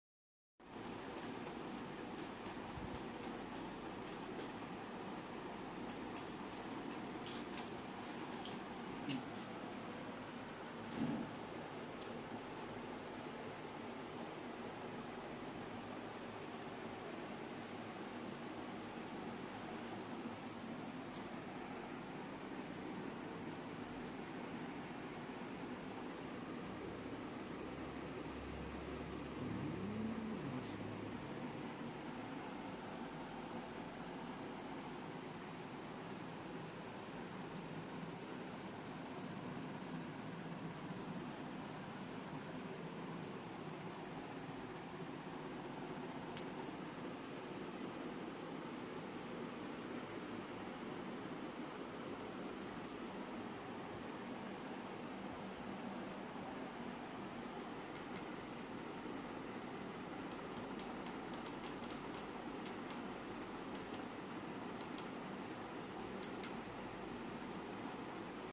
MULTILEVEL INVERTER || Final year project || power Electronics | power System
3 PHASE MULTILEVEL INVERTER (in 5 level ,7 level ,9 level ) . All in a single kit.